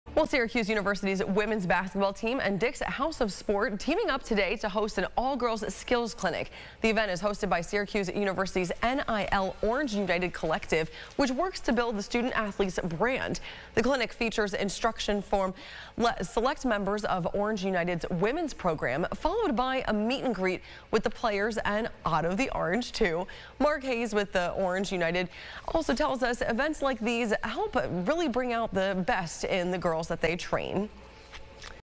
Syracuse University’s Women’s Basketball hosts ‘Girls Skills Clinic’
Syracuse University’s Women’s Basketball Team and Dick’s House of Sport teamed up Sunday to host an all-girls skills clinic.

The event is hosted by Syracuse University’s NIL ‘Orange United’ collective, which works to build the student athletes’ brand.